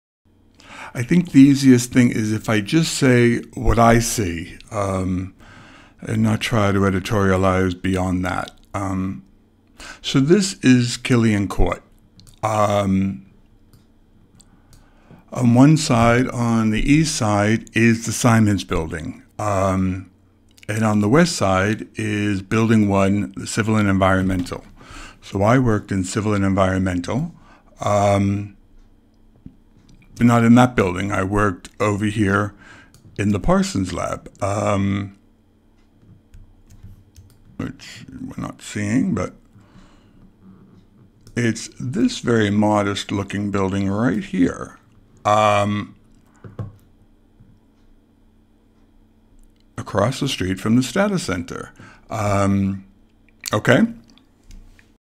Atlas Shrugged @MIT - Part 2
A short video showing an aerial view of MIT’s Killian Court, with the nicely refurbished, privately funded Simons Building on the East Side and the unrefurbished Building 1 on the West side. Then over to Building 48, the Parsons Lab, where I worked as the building manager. The broken communication between these two halves of a single whole is the essential point I want to make, to make the larger point of the broken communication within MIT itself.